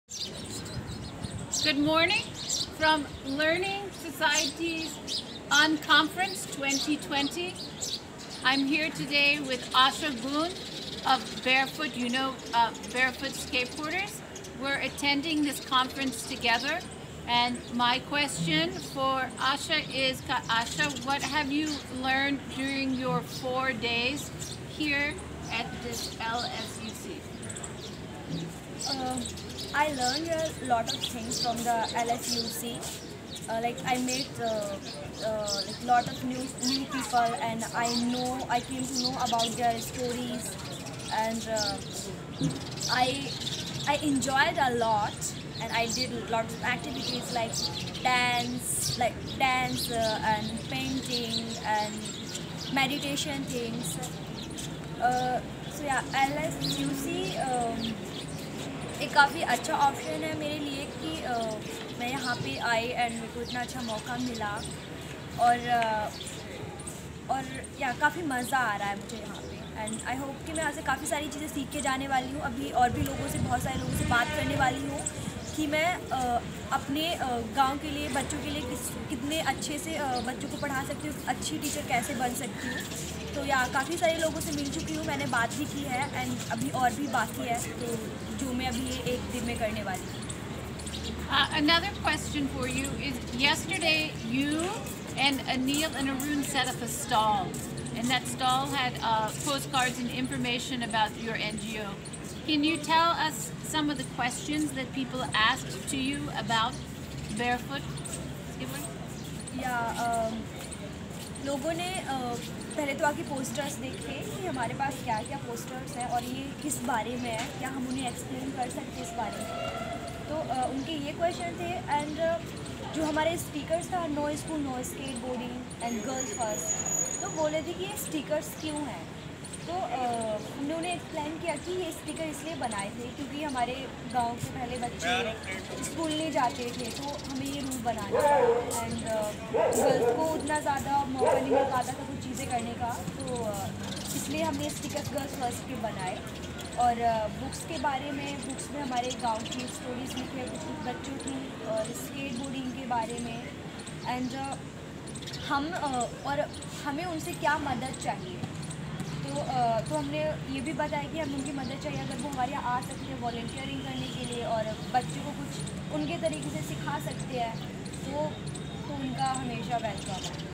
Take aways from LSUC ( Part 1)
Asha talking to Rachael Hammerlein at the Learning Societies Unconference Conference (LSUC).